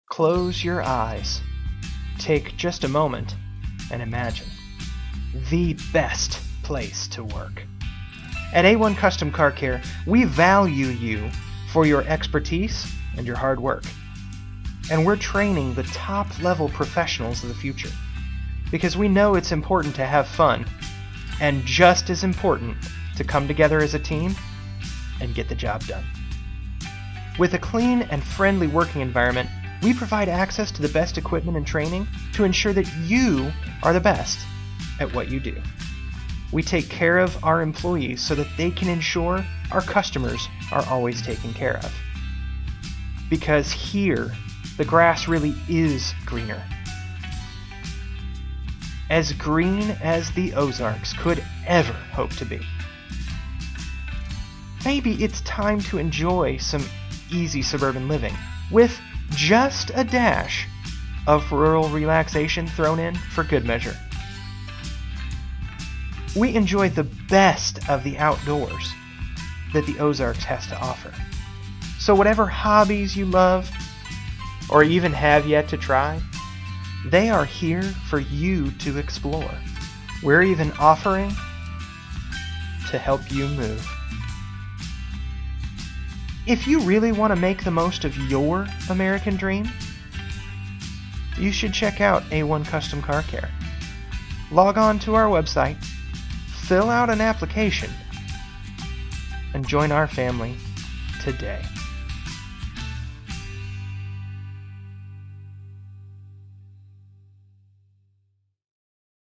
Join the Family at A-1 Custom Car Care
Sometimes you have to make a bold move in life to make things better.  If you are a skilled technician or someone who wants to get into the auto repair business, A-1 Custom Car Care would like to talk to you.  A-1 has three locations in the Springfield, Missouri area, the heart of the Ozark Mountains.  A-1 offers a sign-on bonus, a healthy weekly guaranteed salary, company-paid training, excellent benefits and more.  We'd like to hear from you.